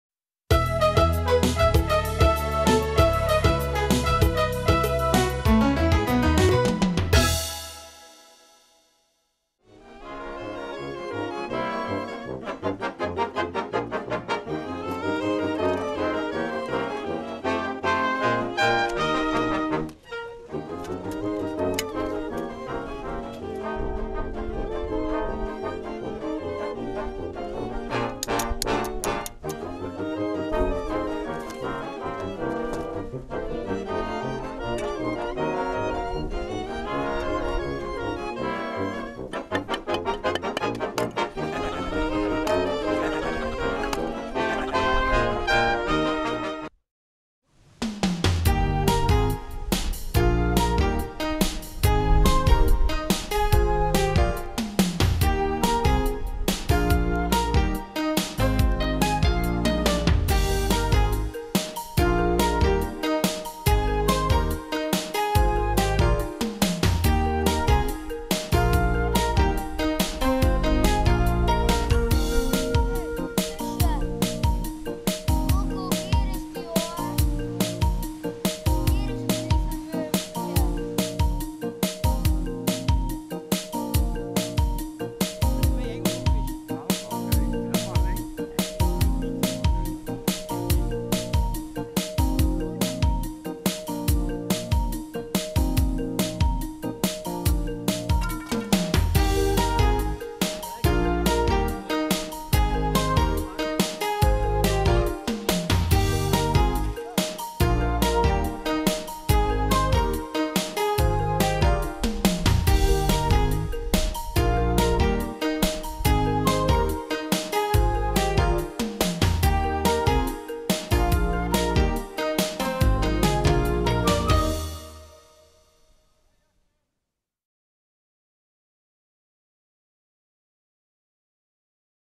2013 06 15 zaterdag vissen
korte samenvatting van het opbouwen en uiteindelijke visevenement aan de Vivaldisingel in samenwerking met activiteitenteam Getsewoud en de Oranjevereniging Beatrix gemaakt door Red Baron Media.